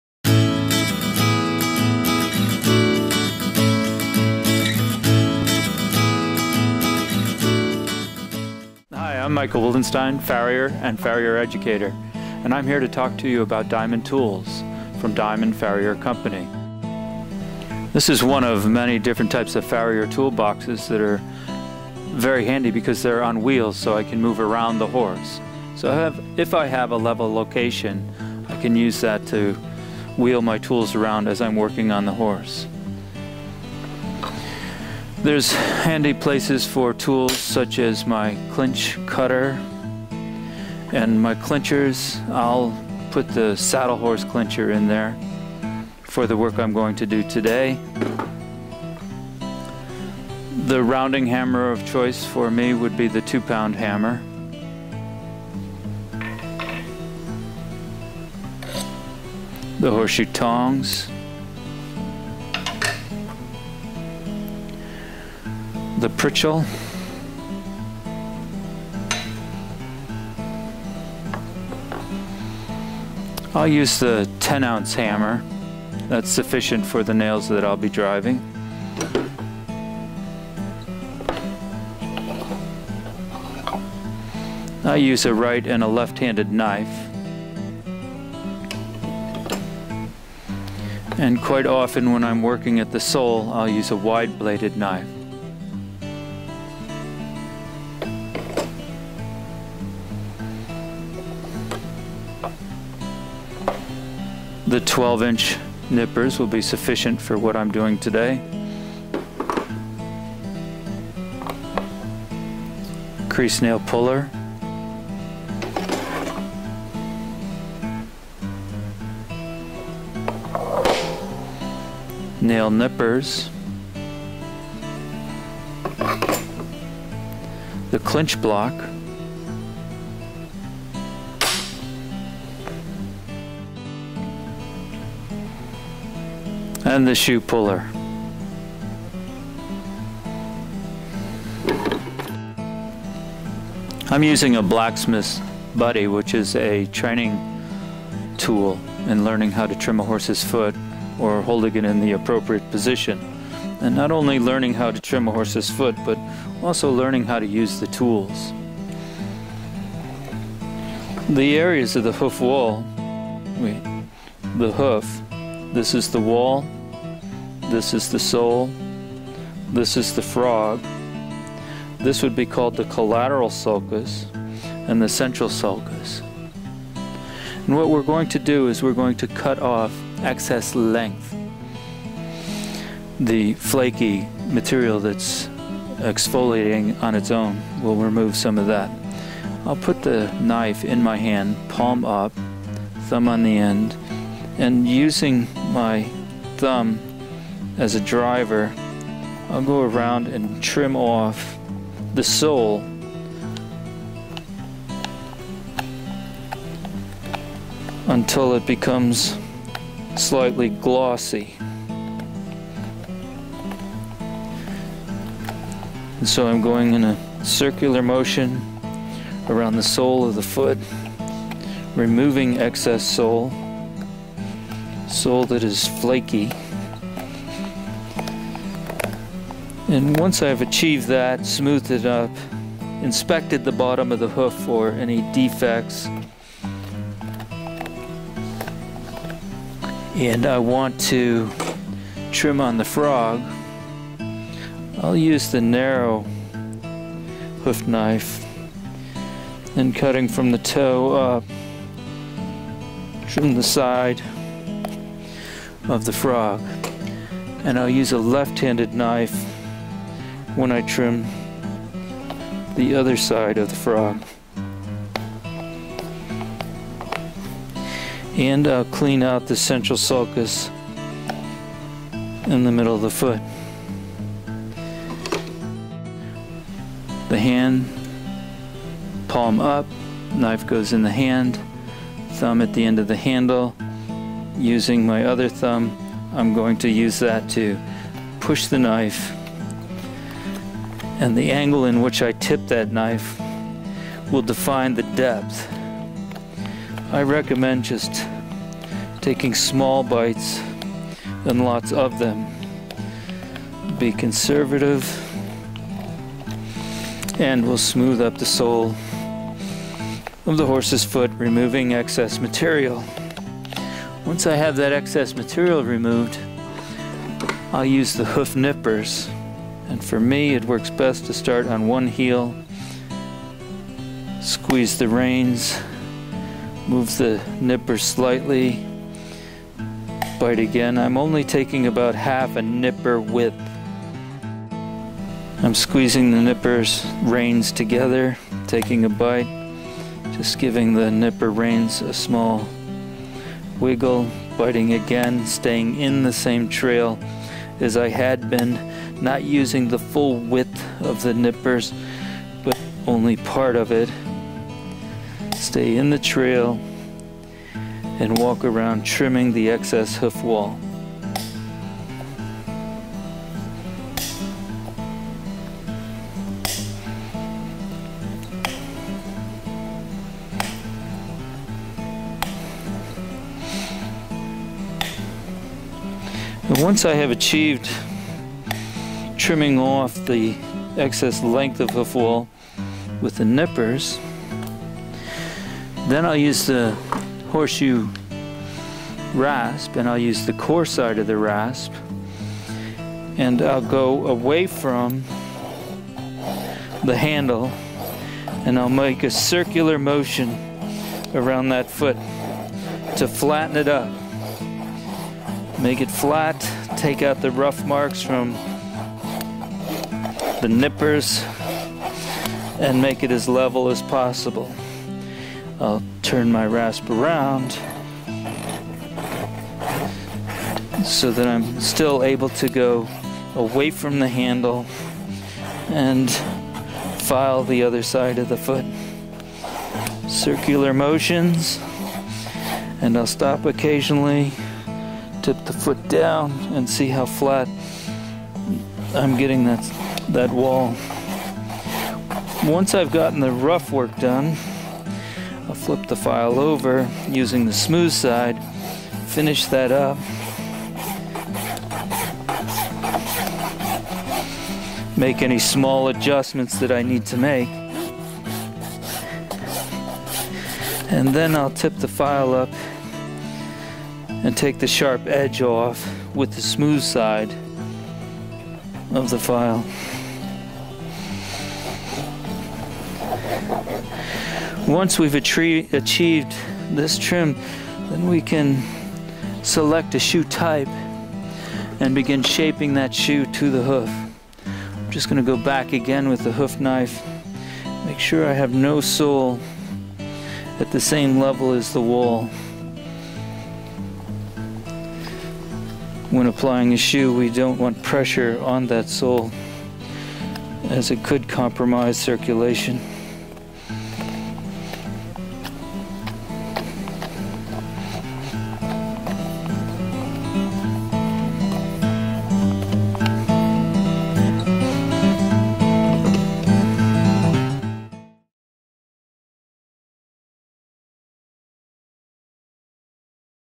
6'' Clinch Cutter Horseshoeing Tool
High quality forged all-purpose tool with one end designed for cutting and the other for pritchel. Beveled on outside edge only. Flush inside edge allows close cutting.